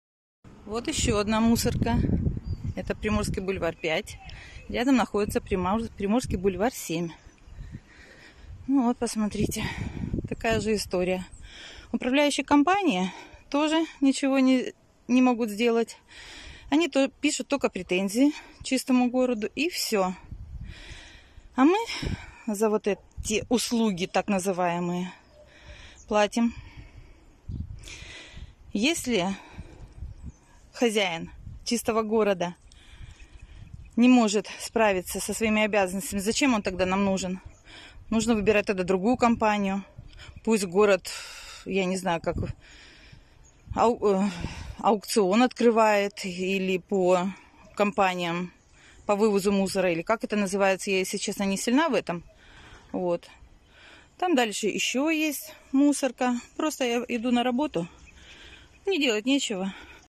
Находка, Приморский бульвар, 5
Жители возмущены бездействием управляющей компании, которая не вывозит мусор с контейнеров